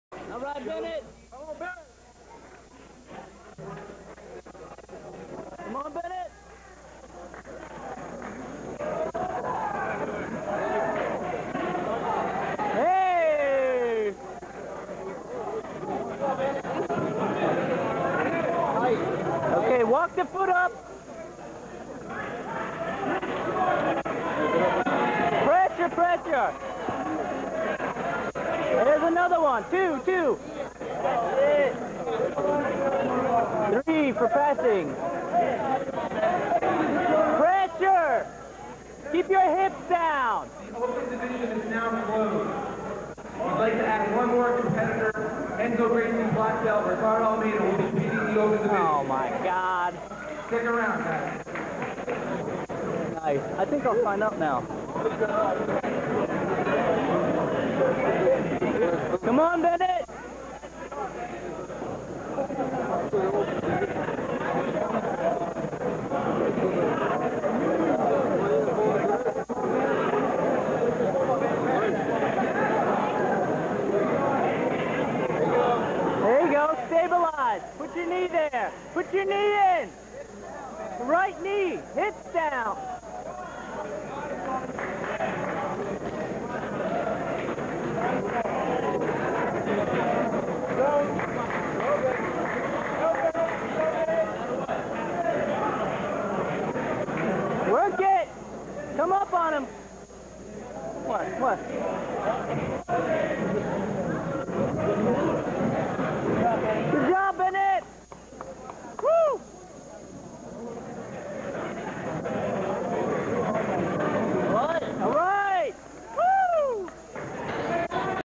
Bennett Fisher vs John Elsiah - White Belt BJJ Match
Bennett Fisher vs John Elsiah in the semi-finals of the white belt division of the Grapplers Quest: Kimono Kombat Challege - June 17, 2000.